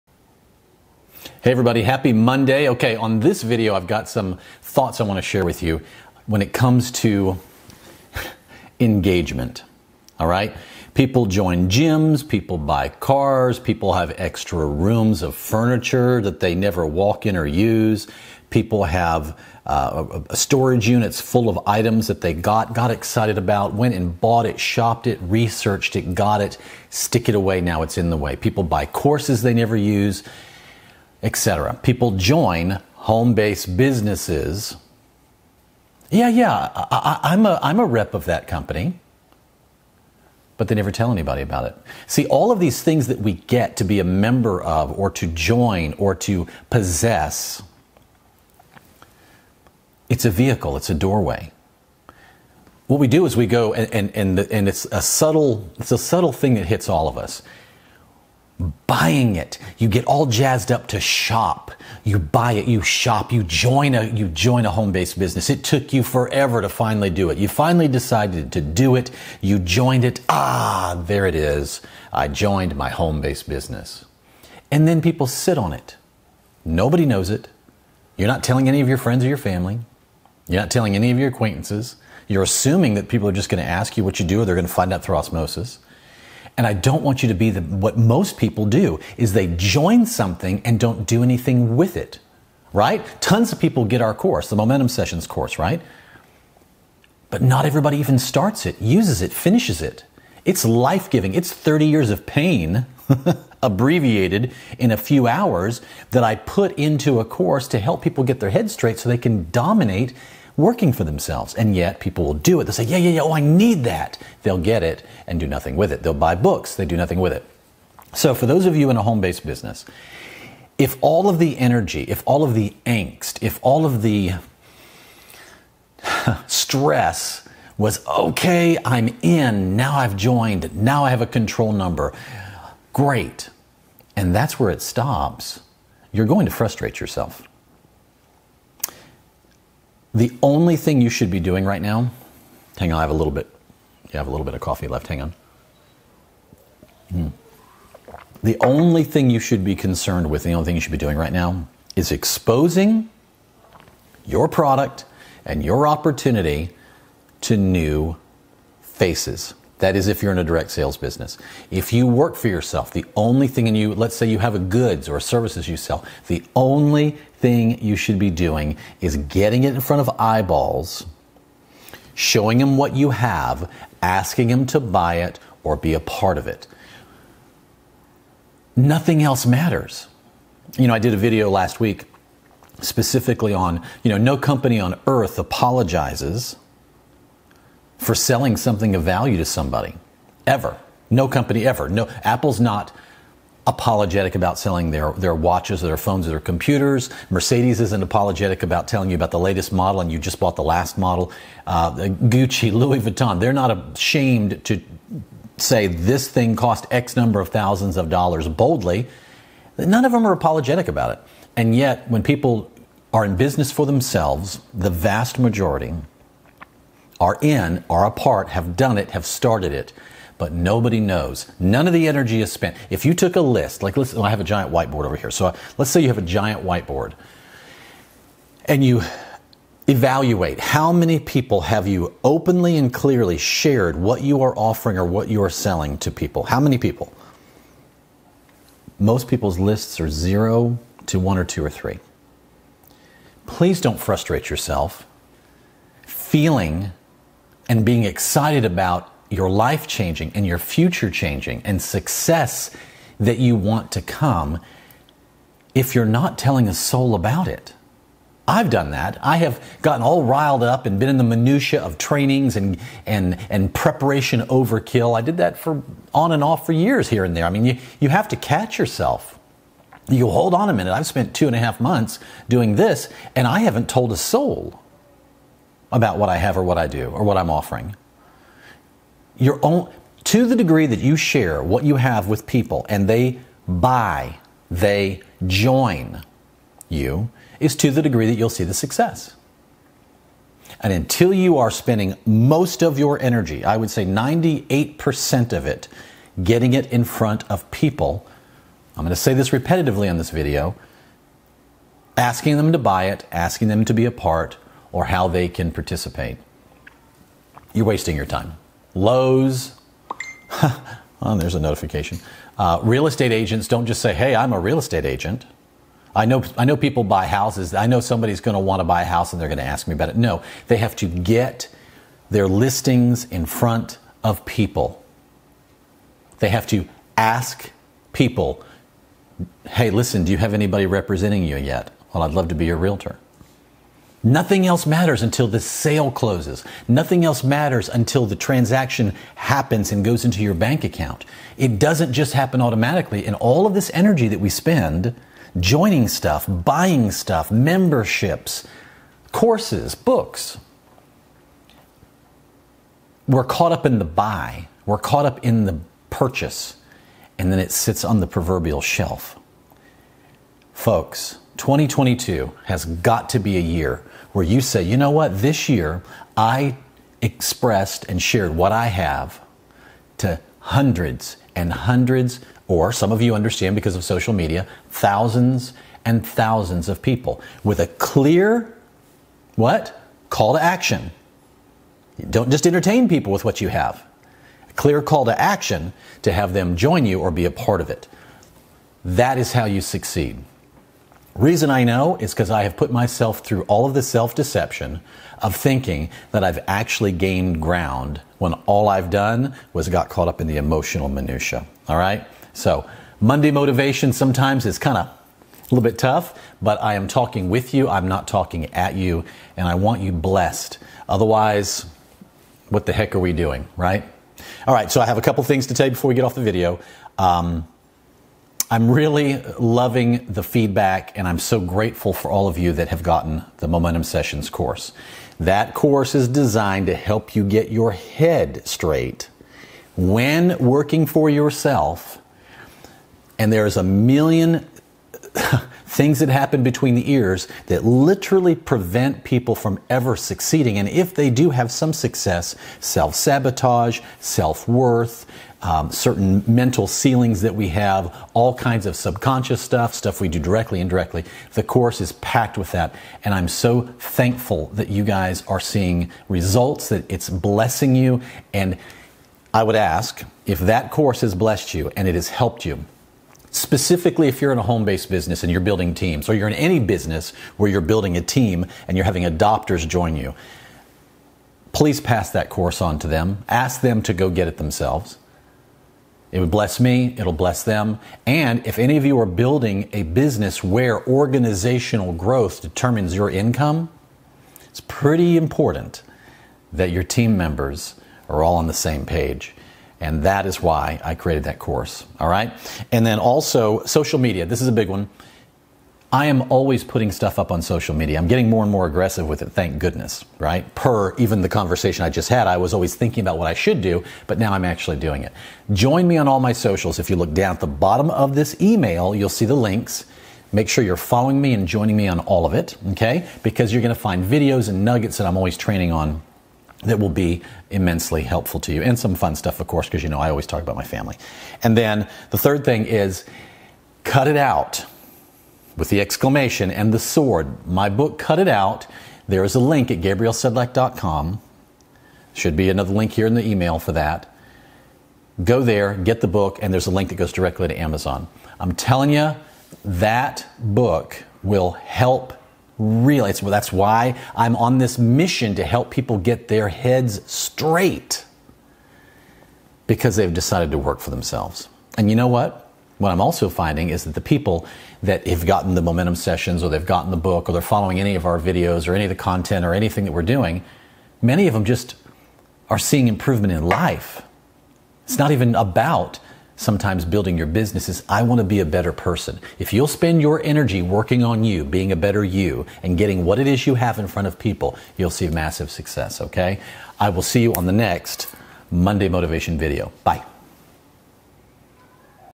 Choosing The Right Network Marketing Opportunity
The major mistake that is made by MANY is to buy, join, or be a member of something and do NOTHING with it. We are a consumer society and all too often the "high" comes from the purchase and the excitement of the "swipe" Then once we get it, THAT WAS IT! The itch was scratched and we are now on the hunt for the next "buy".